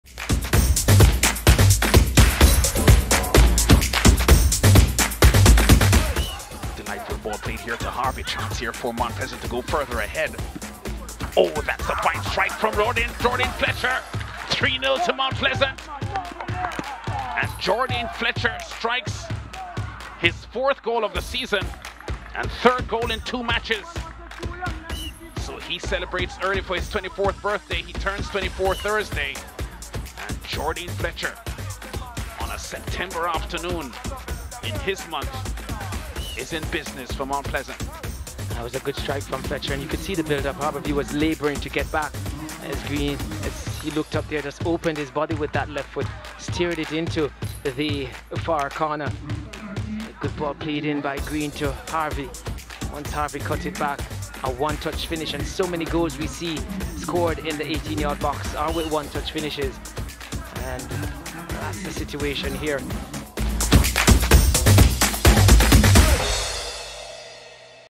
JPL Week 12: Goal of the Week - Jourdaine Fletcher | SportsMax TV
SportsMax TV presents the goal of the week for week 12 of the Jamaica Premier League.